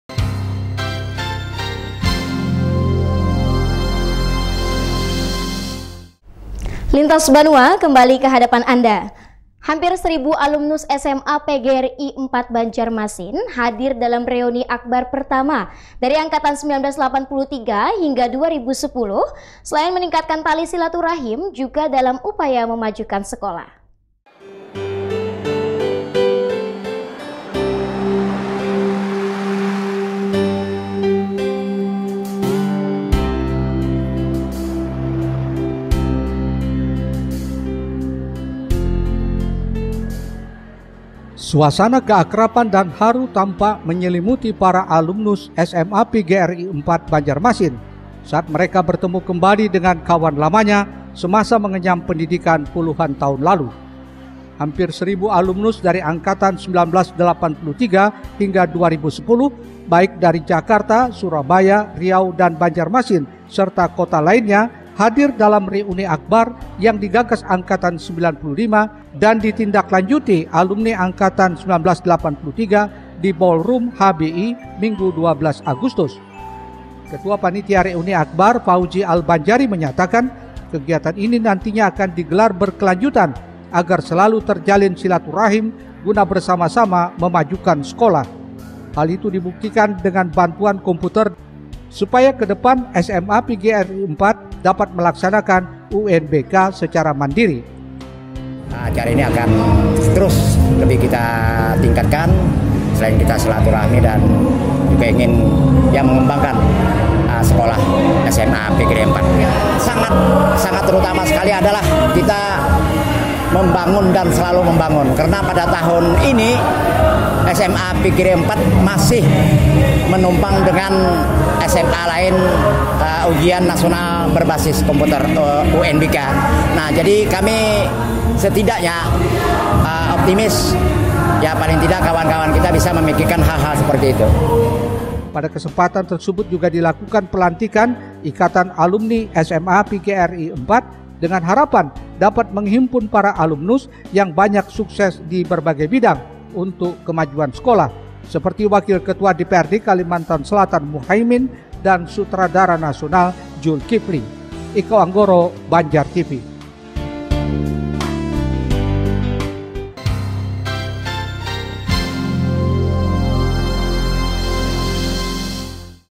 REUNI AKBAR SMA PGRI 4 BANJARMASIN DIHADIRI 900 LEBIH ALUMNUS
Hampir 1000 alumnus SMA PGRI 4 Banjarmasin hadir dalam reuni akbar pertama, dari angkatan 1983 hingga 2010. Selain meningkatkan tali silaturahim juga dalam upaya memajukan sekolah.